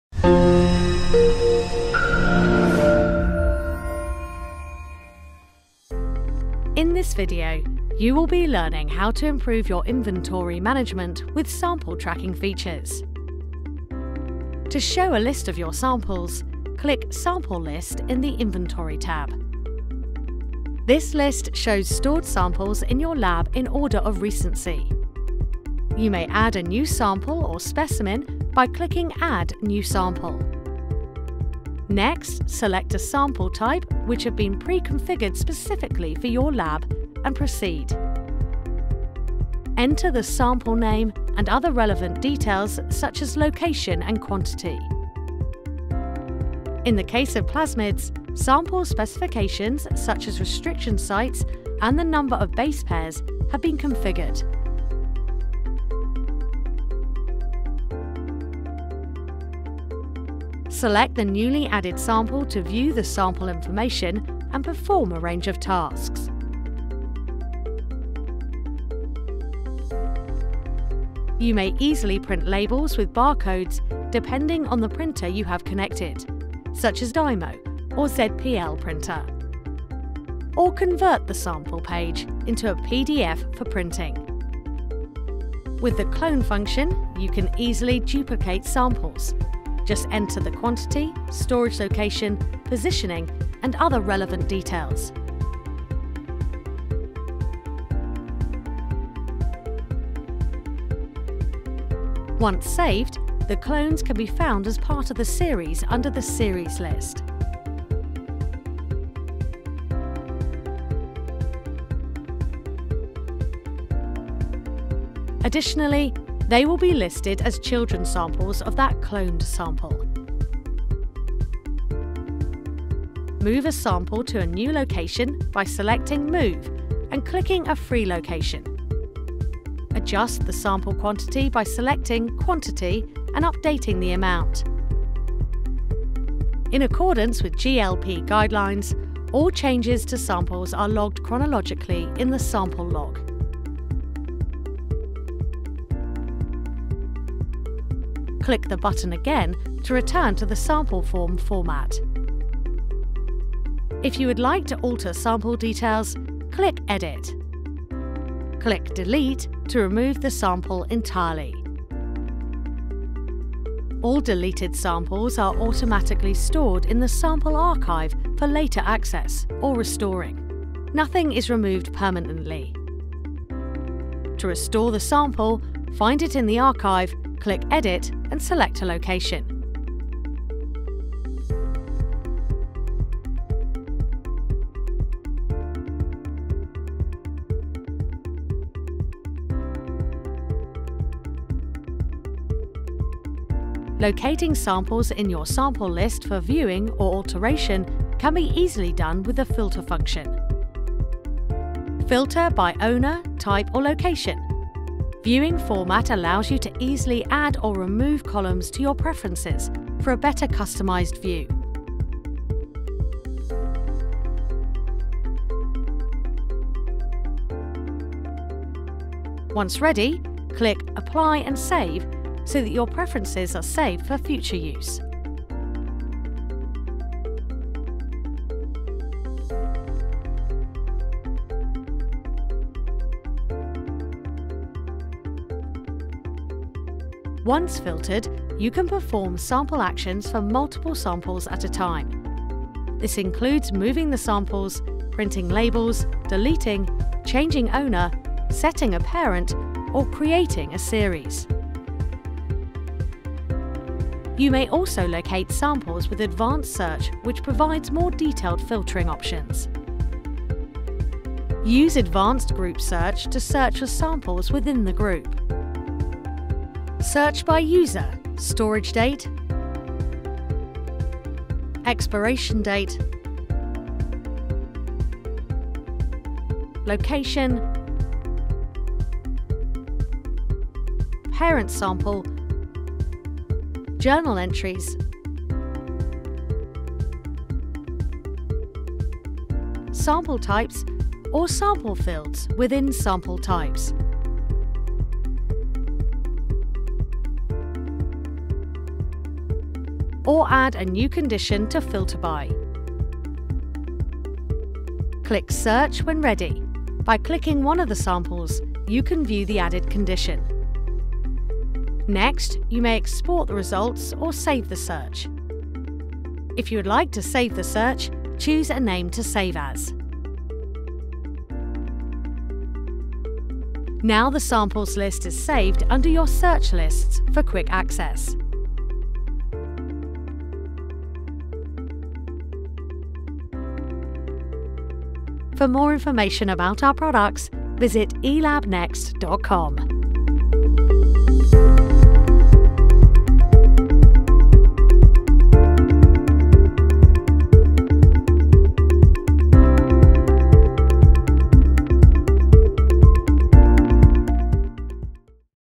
#7 Sample tracking features to improve your sample management | eLabNext
Improve your sample inventory management with sample tracking features in eLabJournal or eLabInventory.

for more information.

00:00 - Introduction
00:14 - Start/View sample list
00:25 - Add new sample
01:00 - View newly added sample
01:10 - Perform a range of sample actions
02:43 - Find deleted samples in sample archive
02:53 - Restore a deleted sample
03:13 - Easily search for samples with filtering
03:57 - Perform actions for multiple filtered samples
04:13 - Use advanced search
05:00 - Add a new filter condition
05:11 - Export or save search results
05:35 - Thank you for watching!